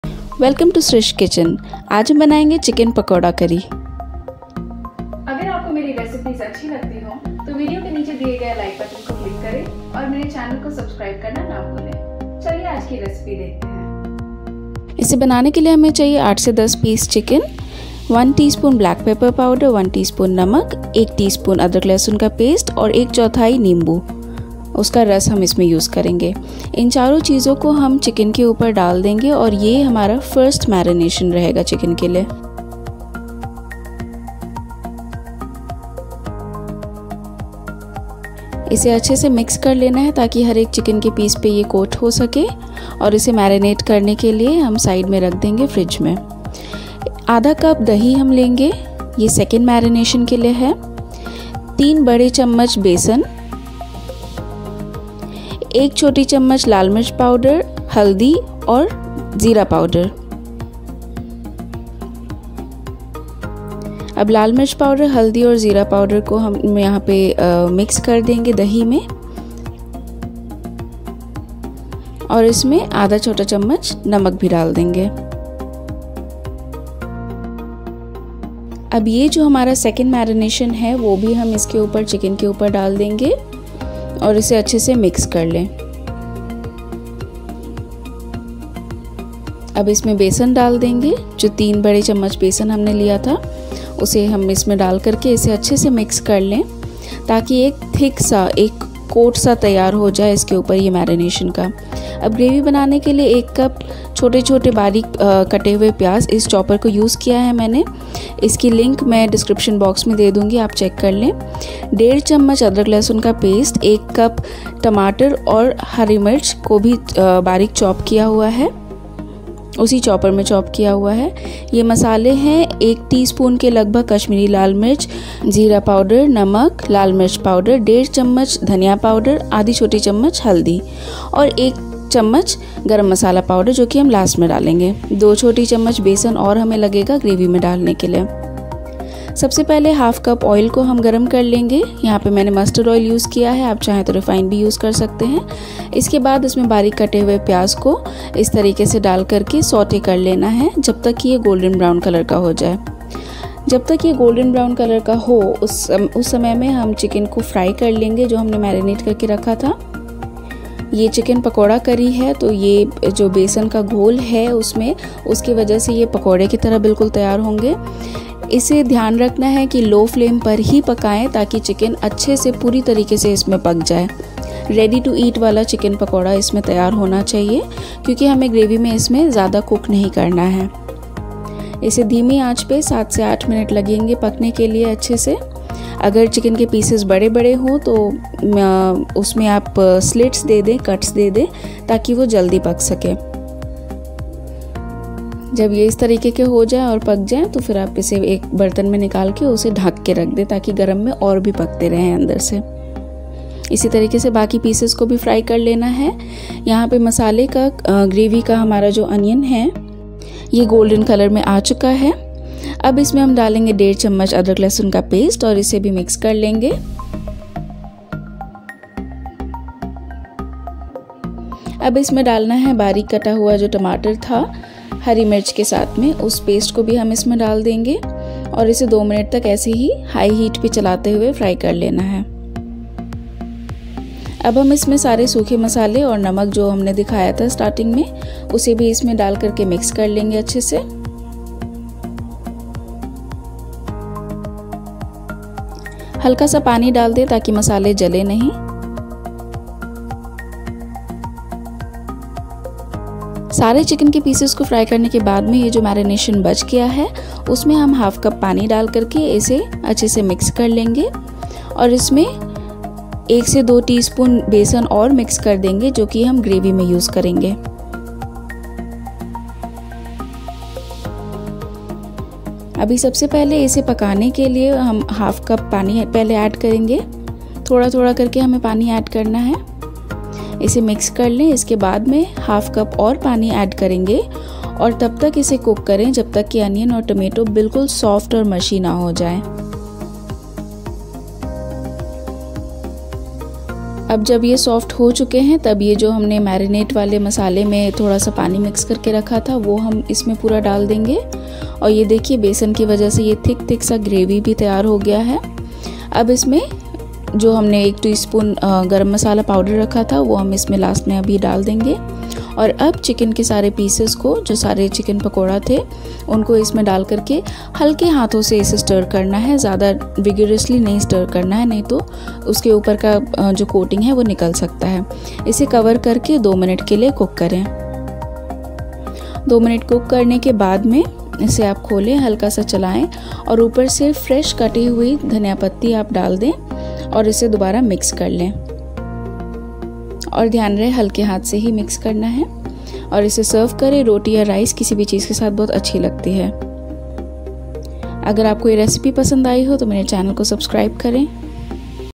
New Chicken Curry Recipe | CHICKEN PAKODA CURRY RECIPE
Are you bored of making all the varieties of chicken curries like, butter chicken, korma, kolhapuri, coriander chicken, lababdar etc etc etc. There are so many ways of making a chicken curry and here is a new way of making an awesomely delicious chicken curry where chicken is first fried into chicken pakodas (which in itself is very very tasty) 😋

Quickly watch the whole recipe and follow it step by step to have the same amazing tasting chicken pakoda curry.

Ingredients list is given at the end of video do check it.

Also, comment and let me know your views about this recipe

chopper link:

New ways of making chicken curry
Restaurant style chicken curry
Thick gravy chicken curry
Rich gravy chicken curry